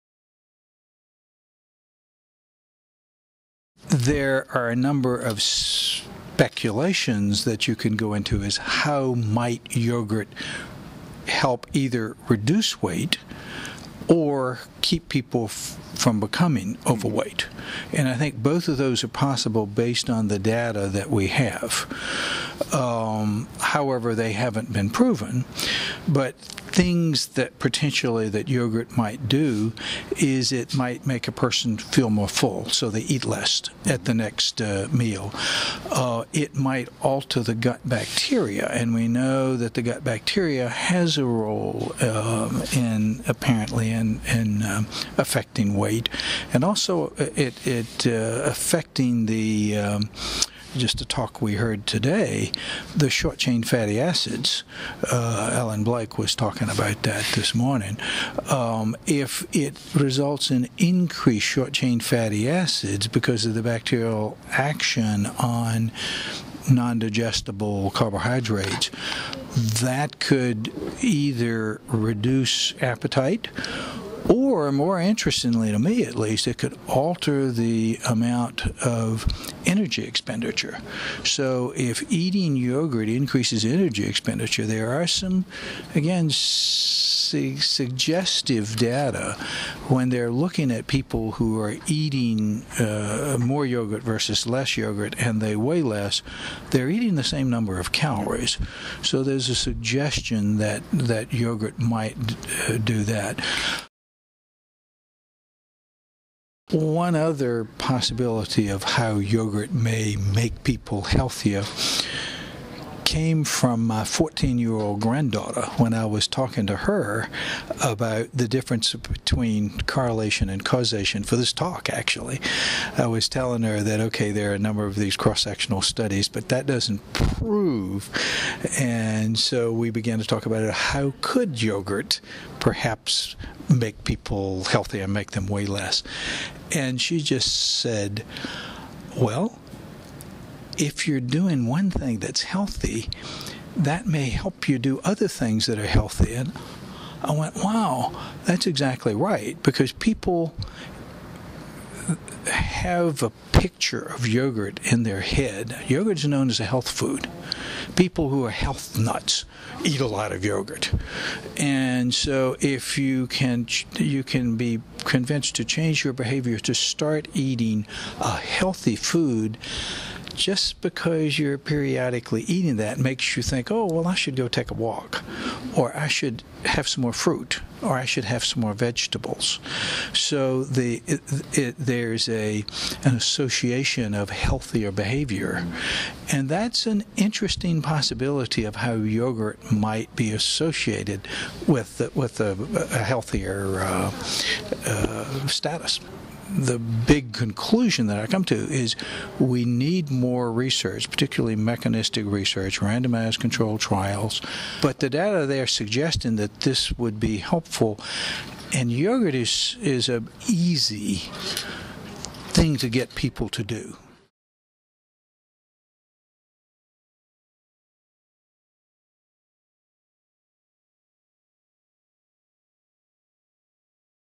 Prof. Richard Atkinson: “debating yogurt as a public health benefit for weight management”
Prof. Atkinson talks extensively about his systematic review on yogurt and weight management. Main learning: there's a lot to learn from the literature!
Share your comments under this video and on Twitter using